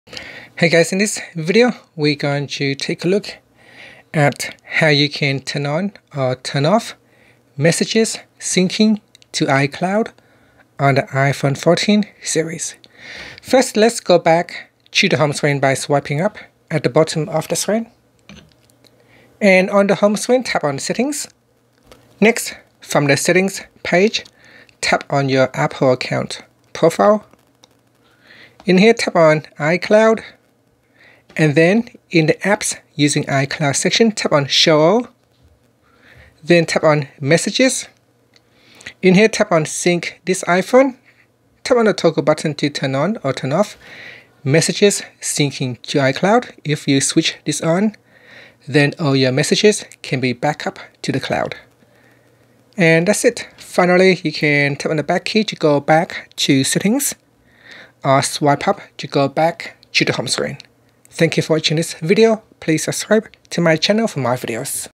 iPhone 14/14 Pro Max: How to Turn On/Off Messages Syncing To iCloud
Learn how you can turn on or off messages syncing to iCloud on the iPhone 14/14 Pro/14 Pro Max/Plus.